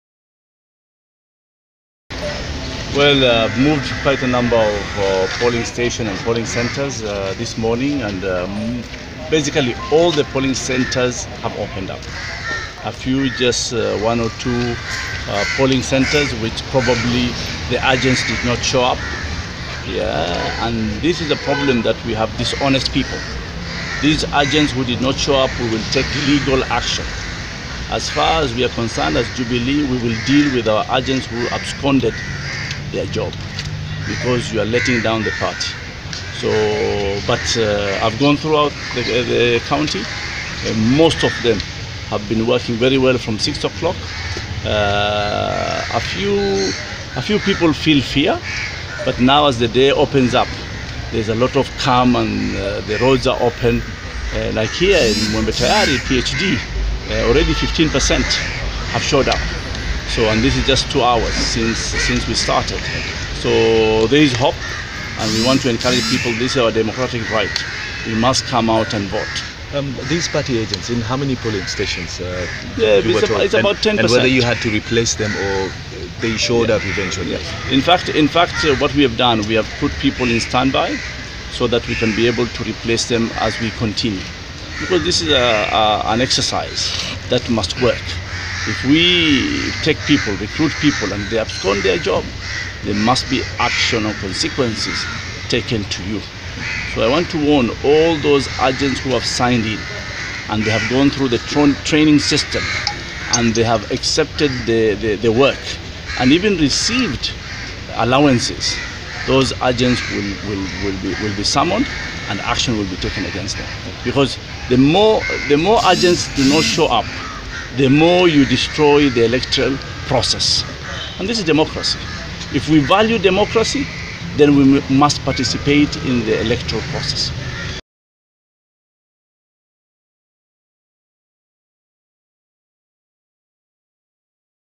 Balala condemns Jubilee agents for Mvita & Kisauni no show
Tourism CS Najib Balala on Thursday condemned over 40 Jubilee agents who didn't show up partly affecting the party's operations in Mvita and Kisauni.